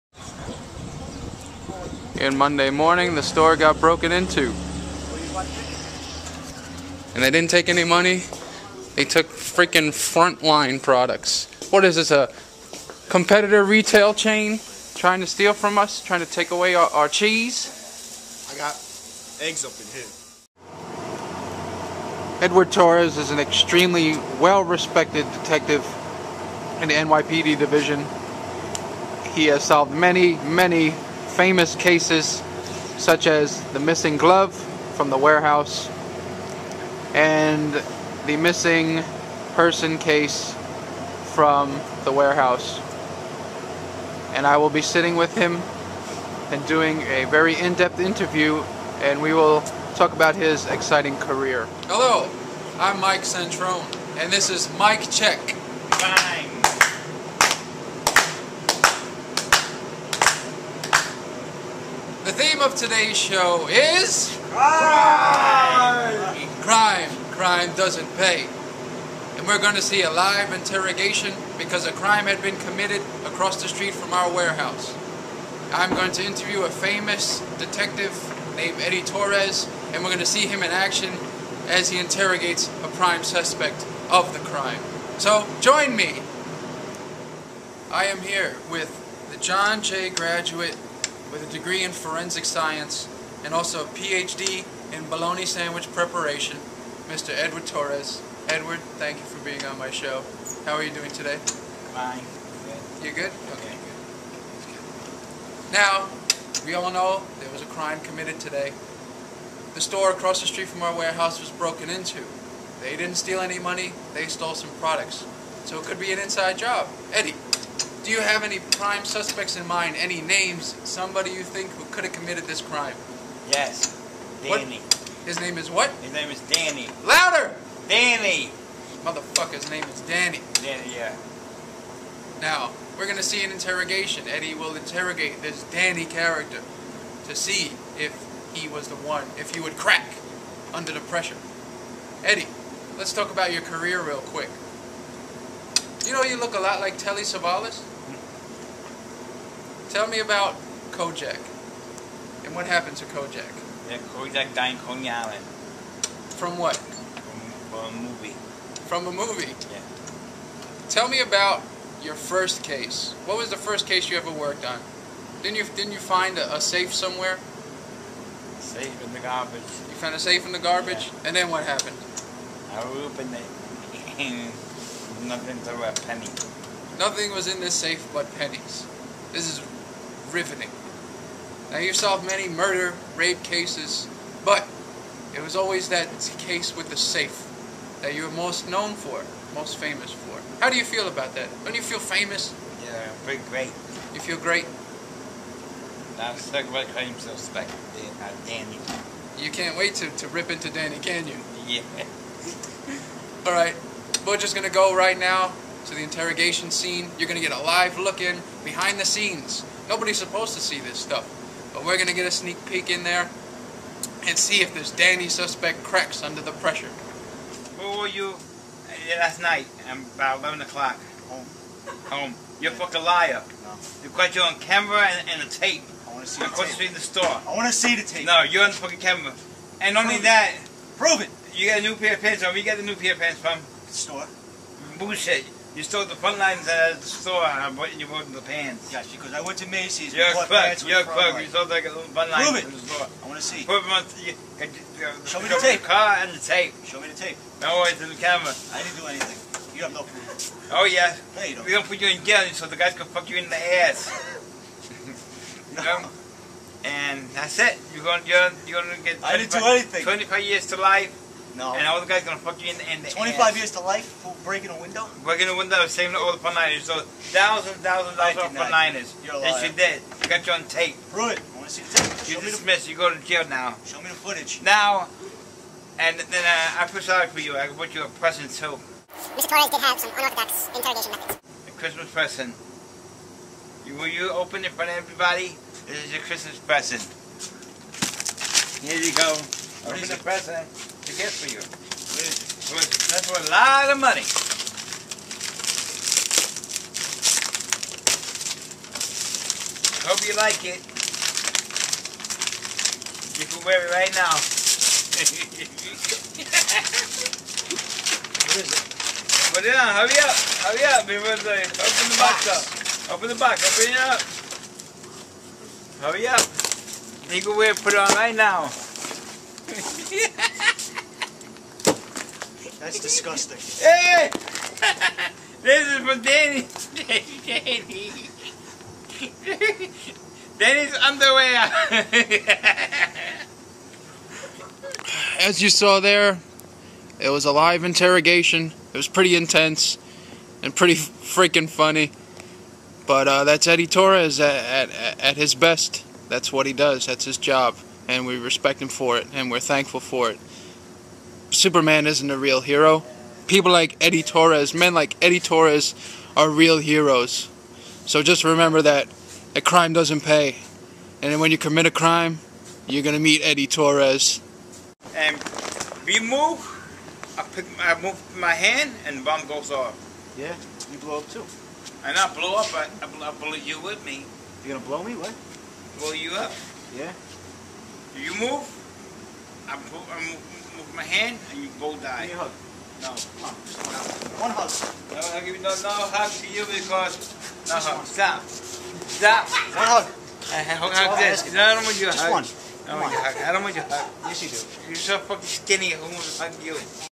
Mike Check Episode 1
"Mike Check" - Episode 1 - "Crime" - A robbery takes place and superdetective Eddy Torres is on the case...